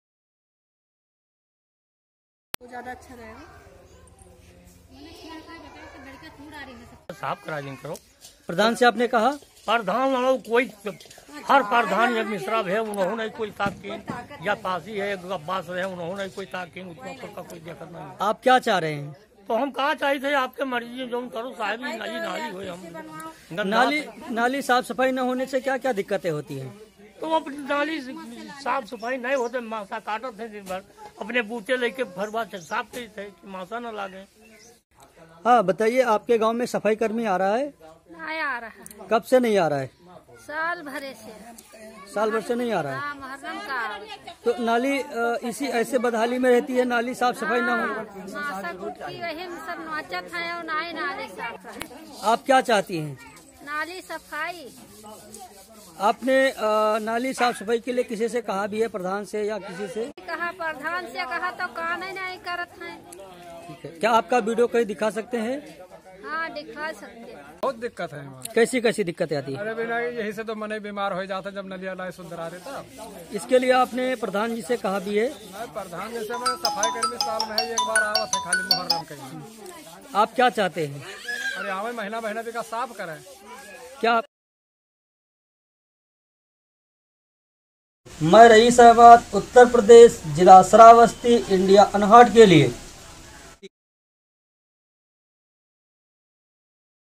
नाली की साफ सफाई ना होने से फ़ैल रही है गंदगी | Sewage Waste is Spreading Due to Lack of Cleanliness
जनपद श्रावस्ती गिलौला ब्लॉक के अन्तर्गत उत्तमपुर गाँव में नाली की नियमित साफ सफाई ना होने से काफी दिक्कतों का समना करना पड़ता है|

In Uttampur village under district Shravasti Gilloula block, there is a lot of problems faced by the lack of clean sanitation of the drain.

Community Correspondent Raeesh Ahmad reports from Shravasti, Uttar Pradesh for Video Volunteers.  Learn more about Raeesh Ahmad and see other videos of this Community Correspondent